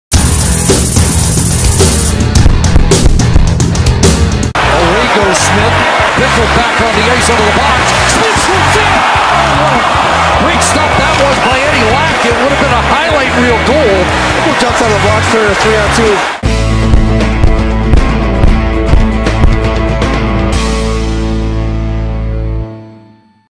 Eddie Lack save vs Chicago Blackhawks | NHL SAVE OF THE DAY (December 20th, 2013)
This video is about the NHL SAVE OF THE DAY on the December 20th, 2013.

Eddie Lack (Vancouver Canucks) made the NHL SAVE OF THE DAY on the December 20th, 2013.